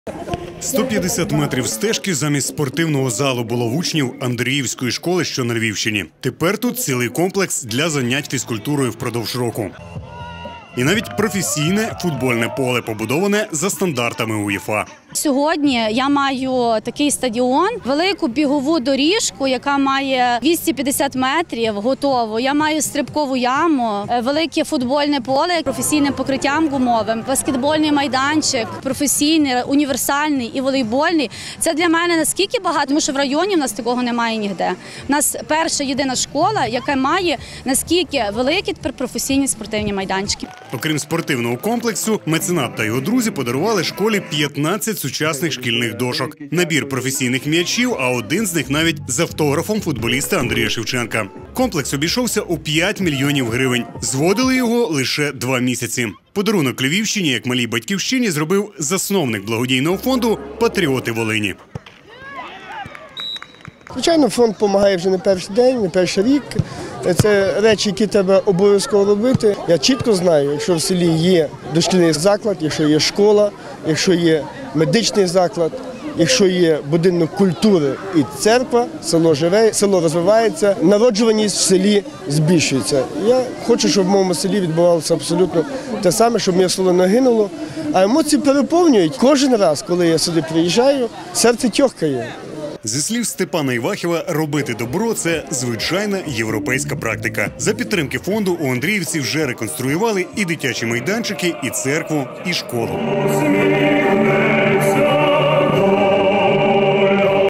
Спорт ціною у 5 мільйонів
150 метрів стежки замість спортивного залу було в учнів Андріївської школи, що на Львівщині. Тепер тут цілий комплекс для занять фізкультурою впродовж року. І навіть професійне футбольне поле, побудоване за стандартами УЄФА.